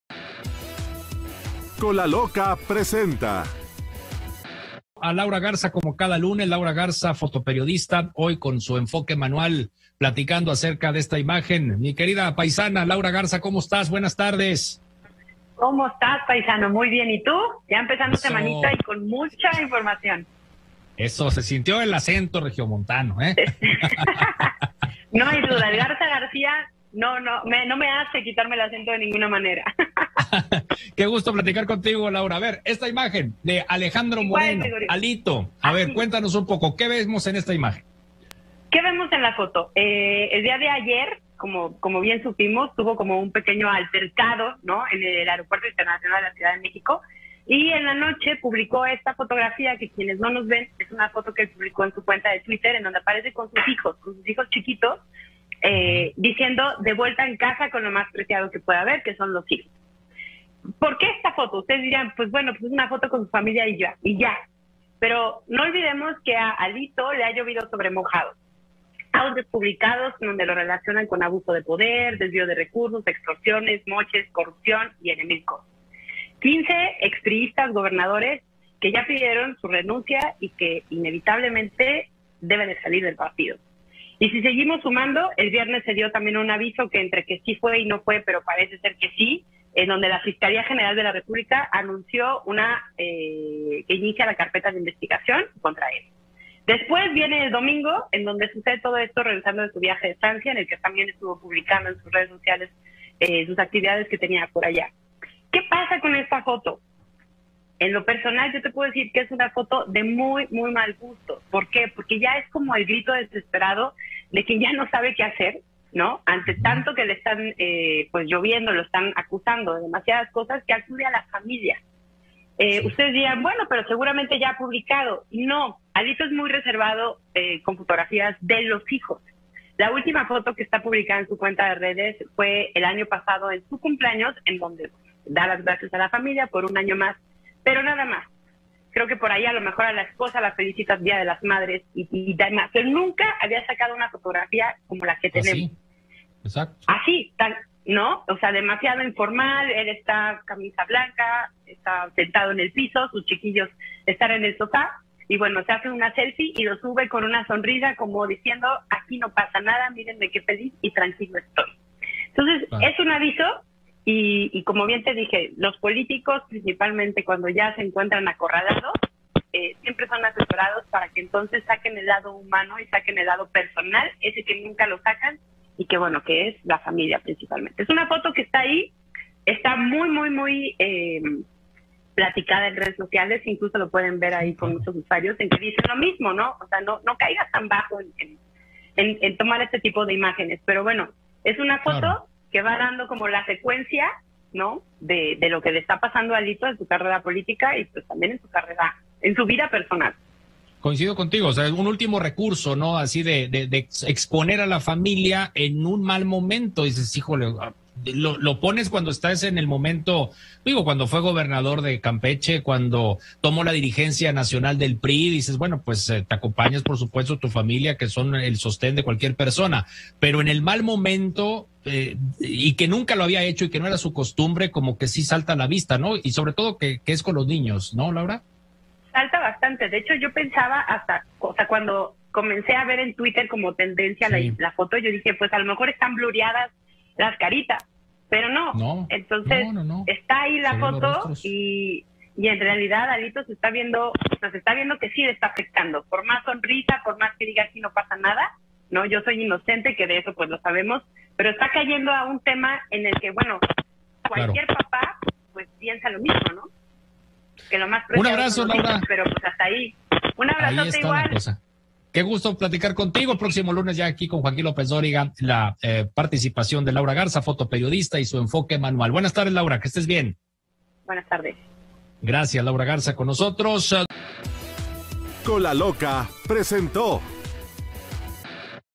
“Alito Moreno publicó una foto de muy mal gusto”: Laura Garza
Laura Garza, fotoperiodista, analizó una imagen de Alejandro Moreno, presidente del PRI, con sus hijos con la leyenda: “De vuelta a casa con lo más preciado que tengo”.
Esta fotografía surge en una coyuntura donde se ha abierto una carpeta de investigación en contra del funcionario, explicó la especialista. “Es una foto de muy mal gusto, no sabe qué hacer y acude a la familia”:
“Alito es muy reservado con respecto a los hijos. Nunca había sacado una foto como la tenemos”, concretó.
Grupo Fórmula